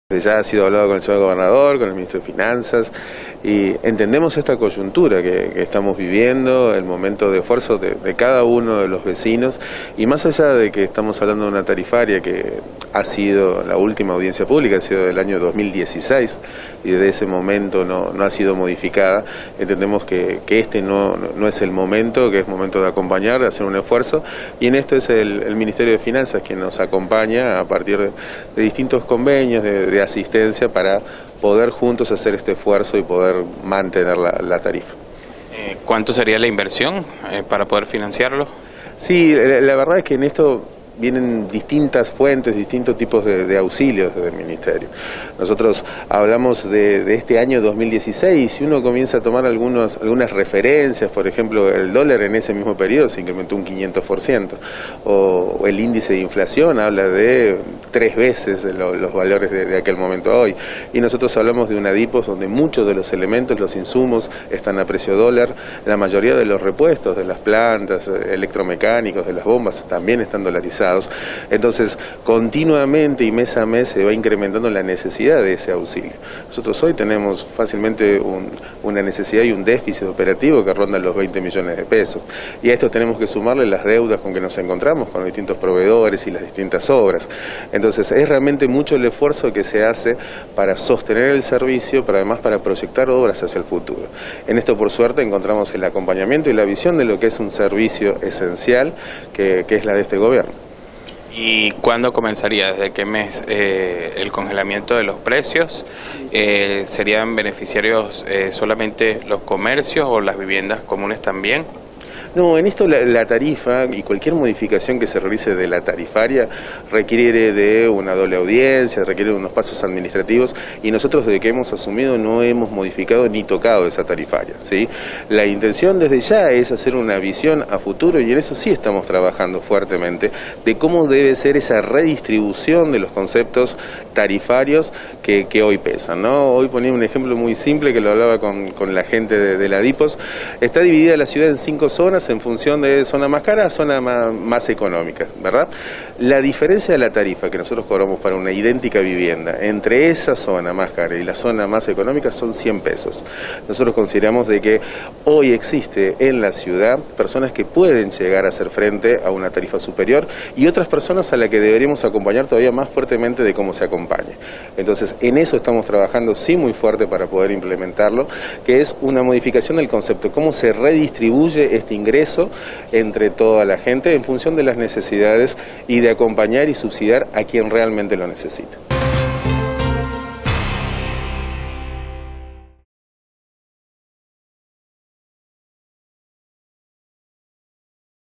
DPOSS mantendrá tarifas congeladas | Gobierno | 21/07/2020 - TV2Noticias
La intención que cuenta con el apoyo de finanzas del Gobierno permitirá ayudar a la ciudadanía.